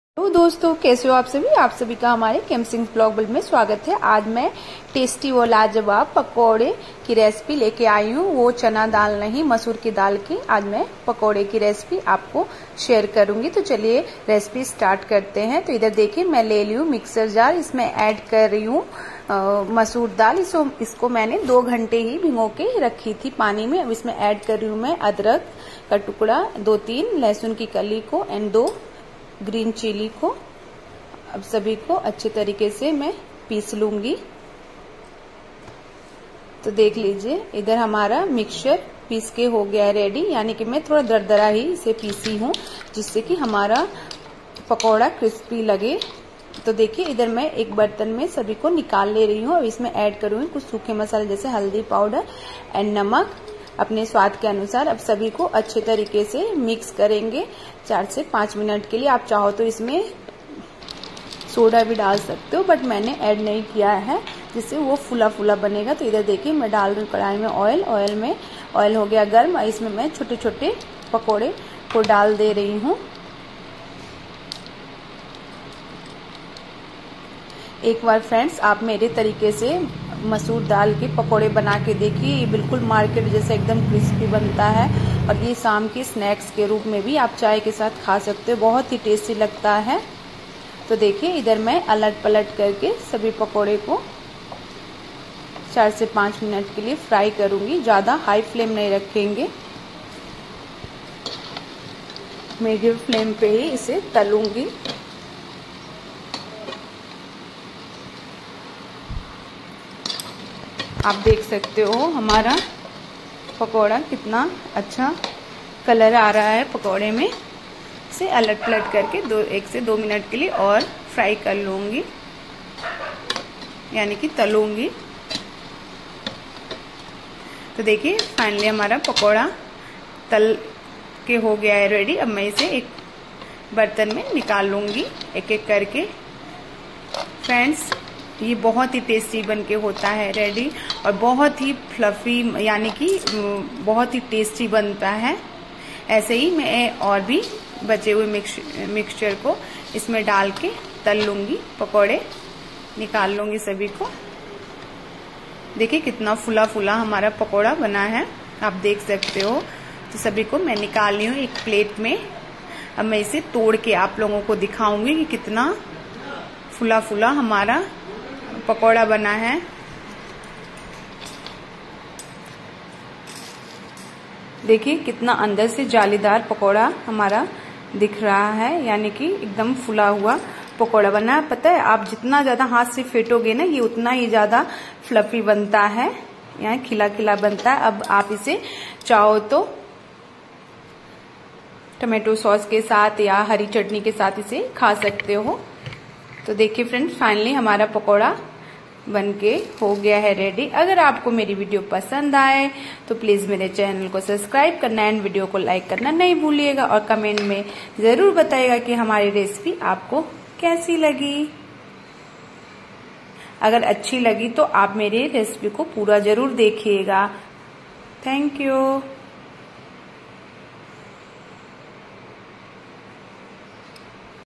Masoor Dal Pakoda Recipe, Red Masoor dal ke Pakode , Crispy Masoor dal Pakoda recipe
Ingredients:-
- Masoor dal
- Onion
- Green Chilli
- Ginger
- Garlic
- Red Chilli powder
- Coriander powder
- Termeric
- Roasted Cumin powder
- Carom seeds
- Salt
- Coriander leaves

- Garam masala
- Oil